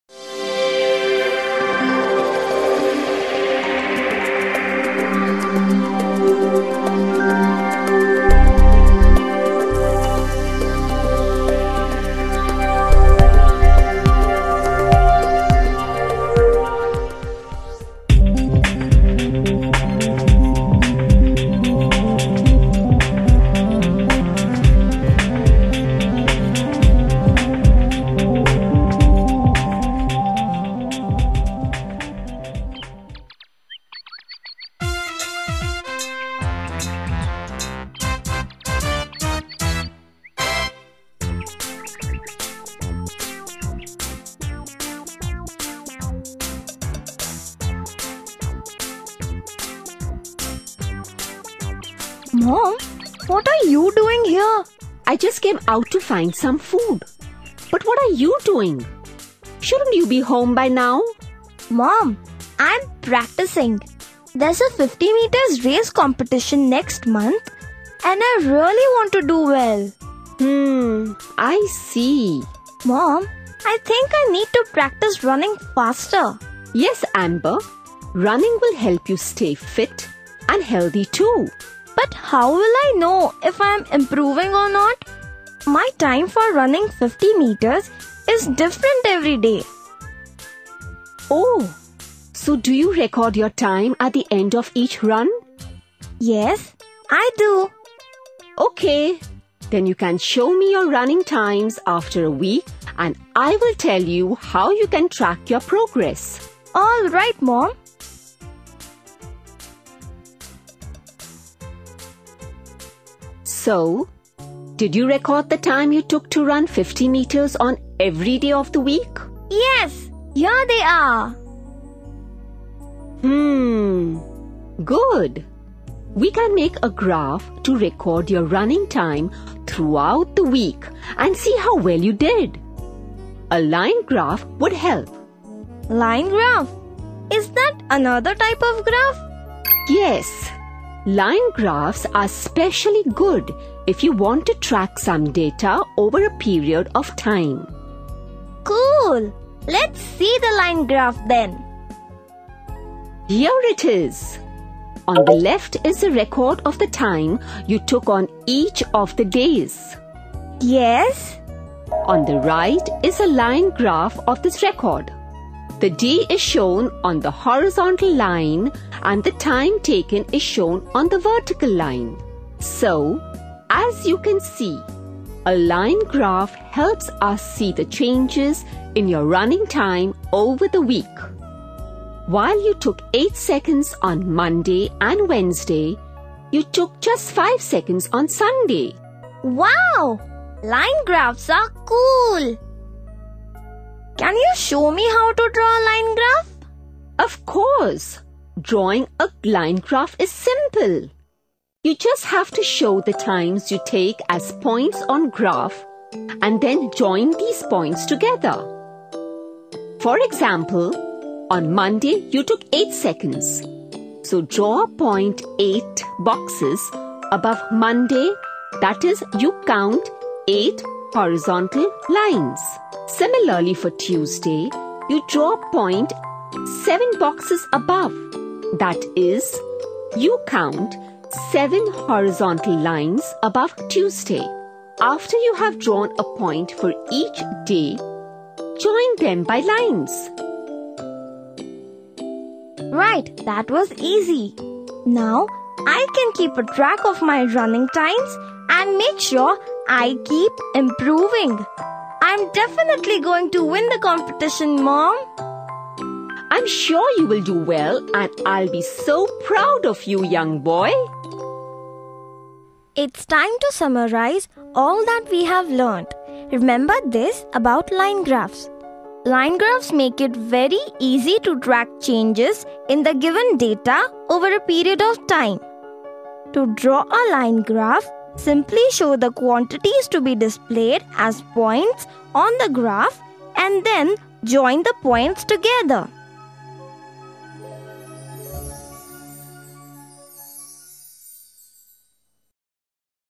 Funbrain: Learn about Line Graphs
You have already learnt about so many form of graphs. In this video you will learn about line graphs which are very important if you track some data over the period of time.
In this Video, Amber prepares himself for the running competition but he was confused about tracking his improvements hence his mom teaches him about the line graph and how he can keep a track of records to see his improvement in the easiest way.
This is the Funbrain mathematics which is very nice to learn!